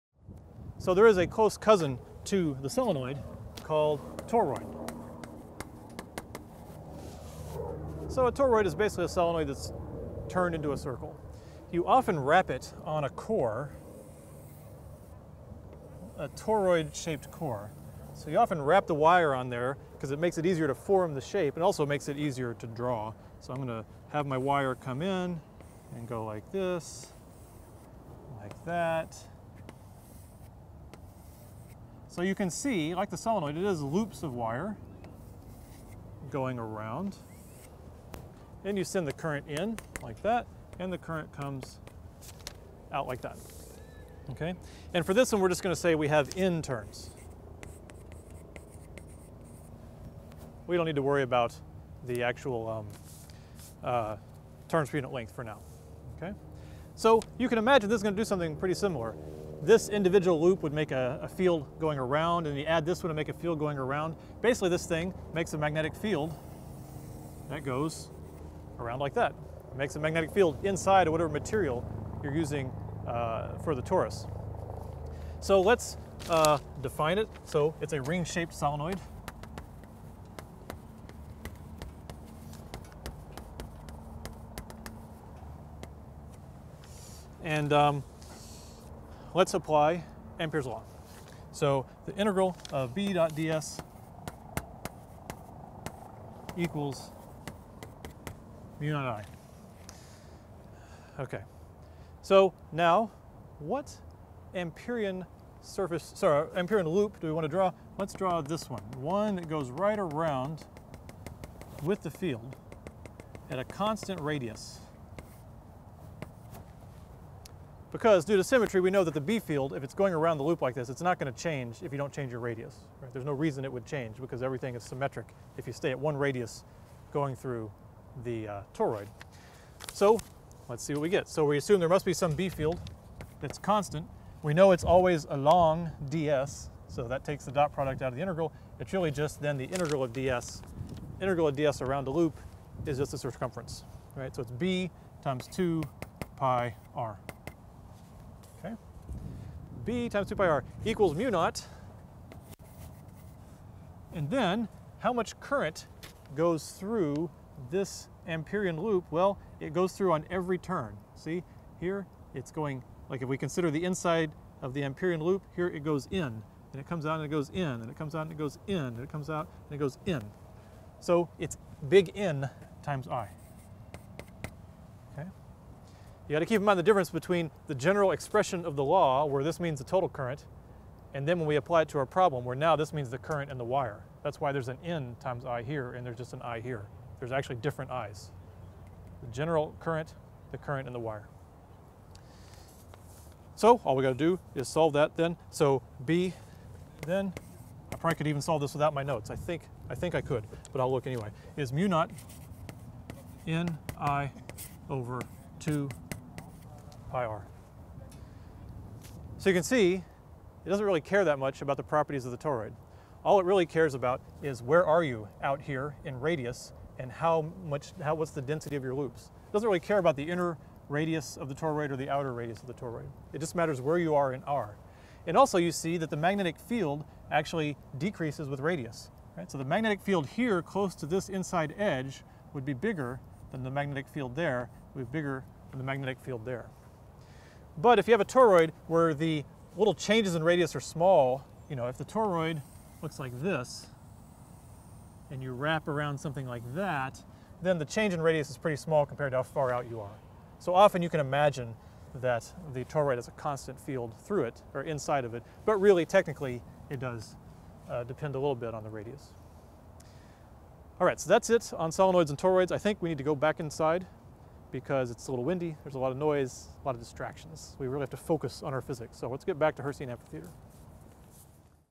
PHYS 102 | Solenoids 4 - Magnetic Fields of a Toroid
Here we introduce toroids and calculate the magnetic field inside.